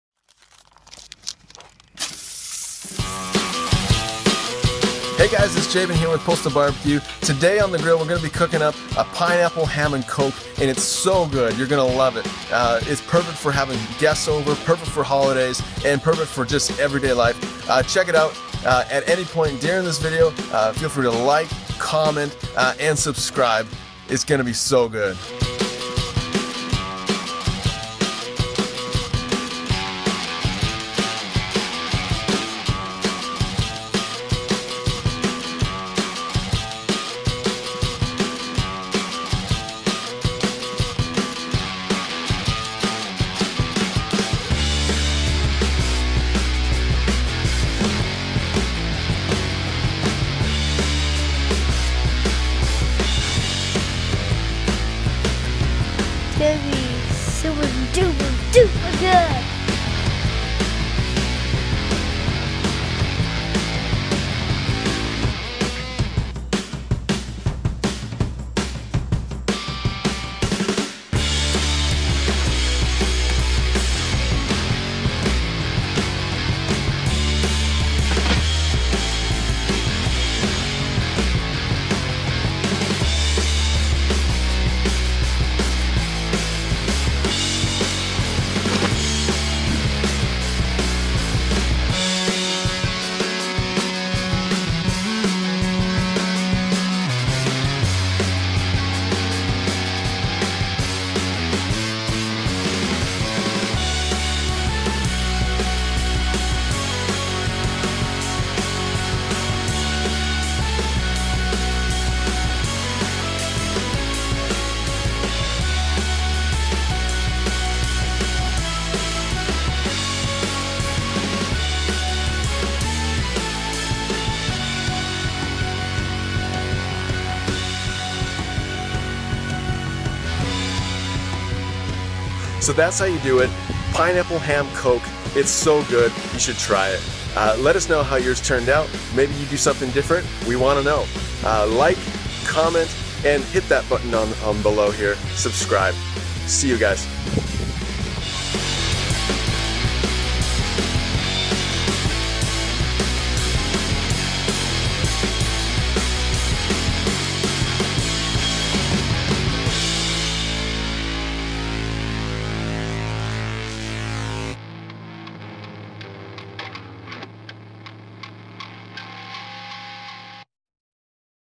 BBQ Ham Recipe | Pineapple Ham with coke glaze
This is a great BBQ Ham recipe perfect for Christmas, Thanksgiving, Easter or everyday eating! TO SEE OUR BBQ ACCESSORY GUIDE

In this Video we want you to experience the most incredible way to cook your traditiona ham.  With sweet and tangy pineapples throughout and a delicious and sticky Coke glaze this will make you drool.  WARNING, this might cause you to dance around like a little school girl who has just saw Justin Beiber.